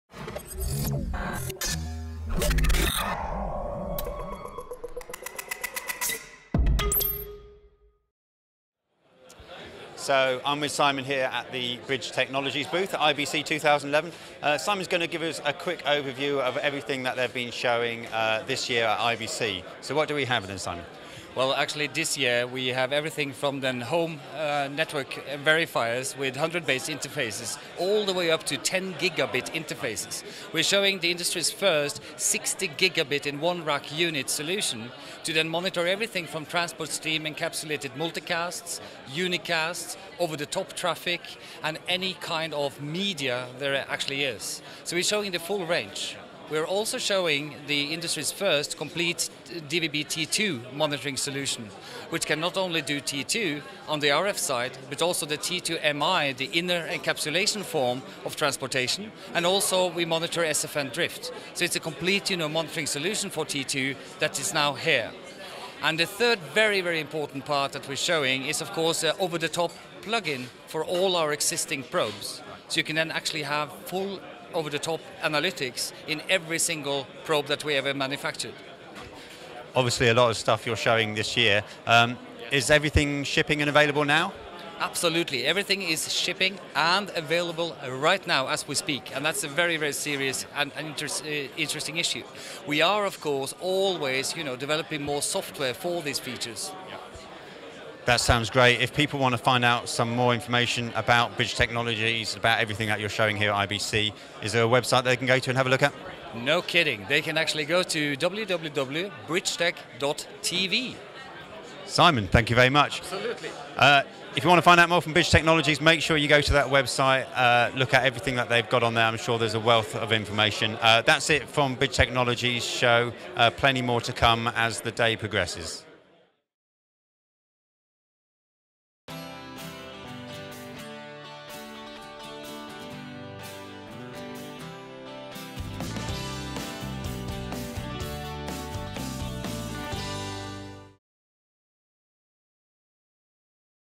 Bridge Tech IBC 2011
Simon Frostad talks about everything from their home network verifier with 100 base interfaces all the way up to the 10 gbit interface together with the industries first 60 gbit in one rack solution to monitor virtually any type of media.